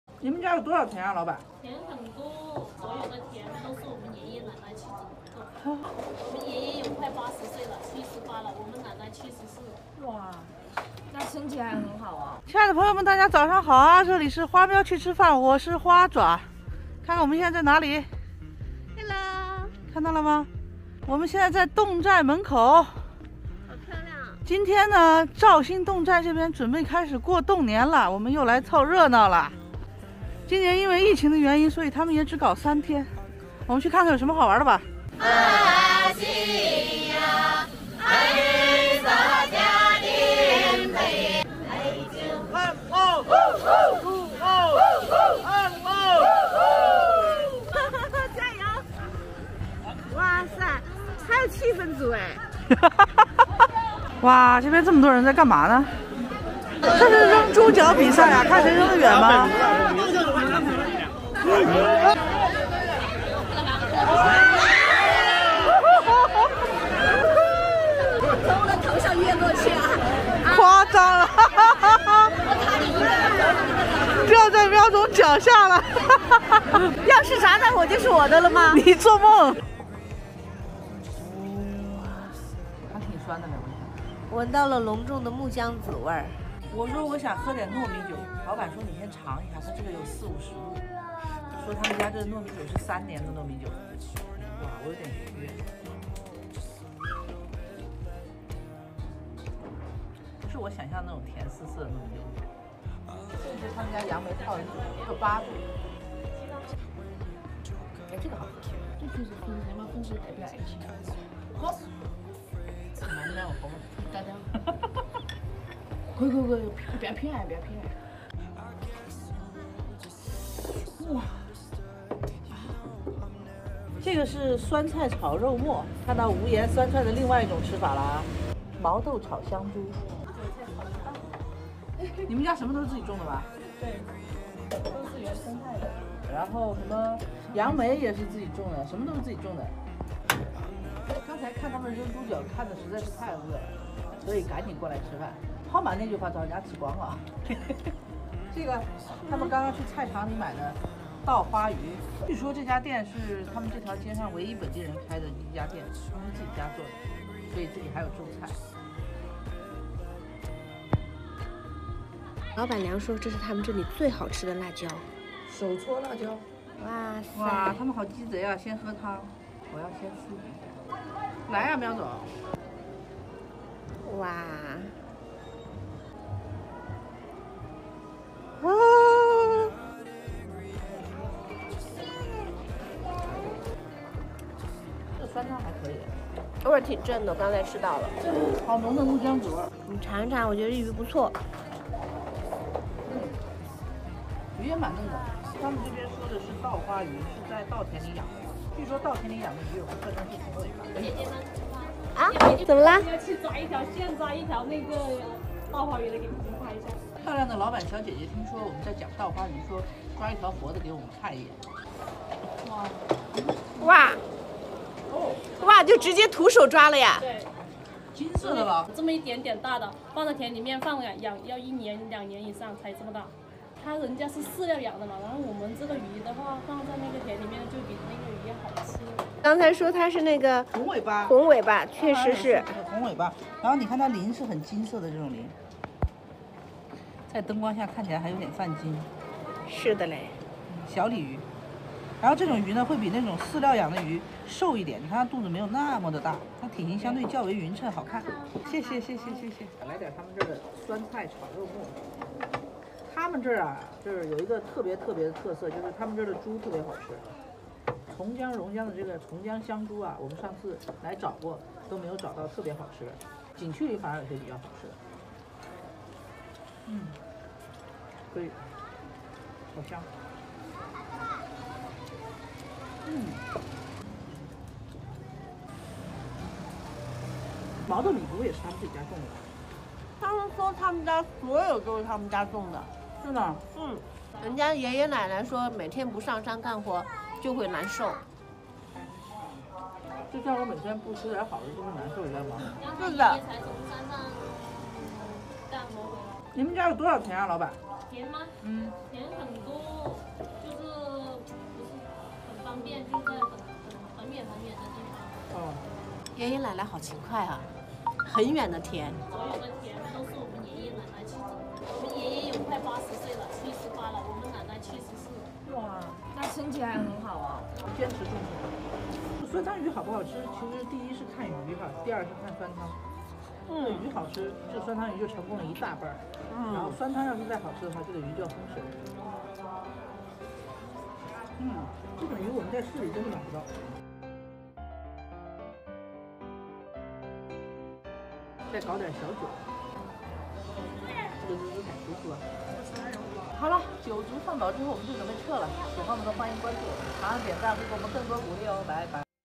侗寨酸汤鱼好绝，70多岁爷爷奶奶相伴种菜，漂亮小姐姐开店卖鱼【花喵去吃饭】
我们找到了肇兴侗寨街上唯一一家本地人卖鱼的店，她们家菜比别的新鲜，也更大份，原来是因为都是爷爷奶奶自己种的，70多岁的爷爷闲不下来，天天跟奶奶种菜去，真美好。